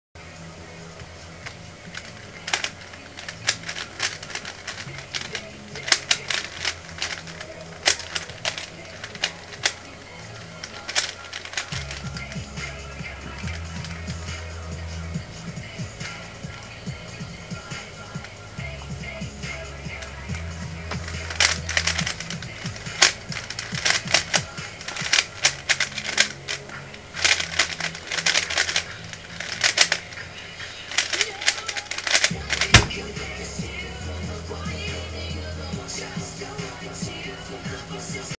11.75 roux solve
B2 D B2 D2 R' U' D' L B' L2 B L2 F2 U' B2 L2 R' D F' U' L U R' D F2

ended with an M2 comm :3